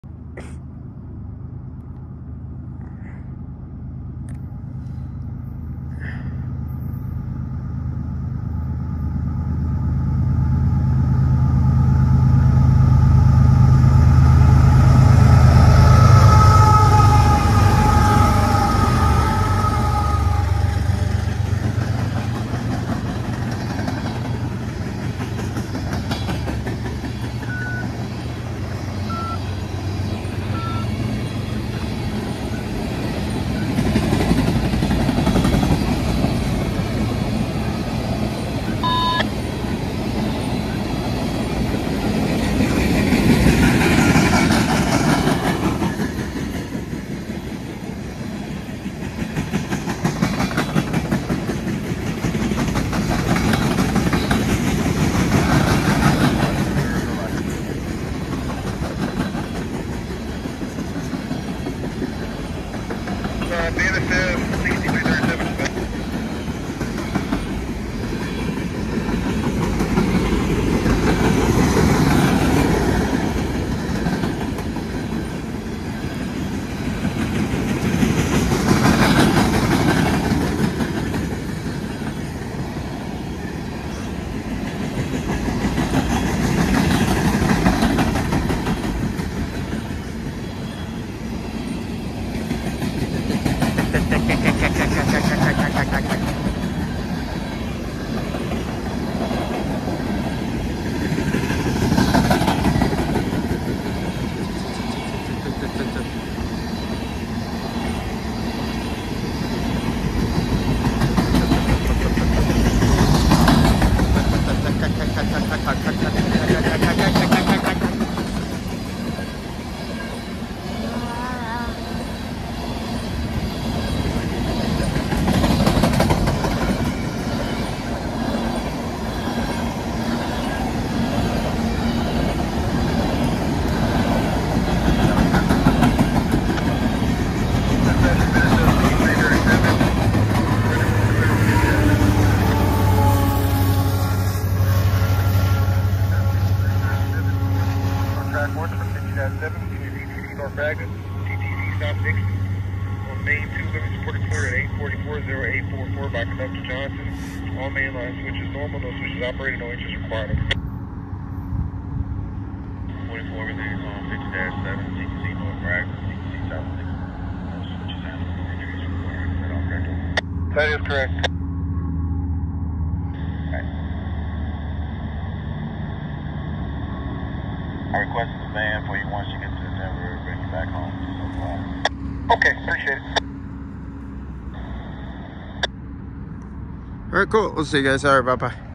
A Northbound BNSF #6337 Empty Energy Coal Train
So Their Was A Northbound BNSF #6337 Empty Energy Coal Train To Denver For The Empty Energy Coal Train.

BNSF
#6337 GEVO ES44AC
#6307 GEVO ES44AC
#6019 GEVO ES44AC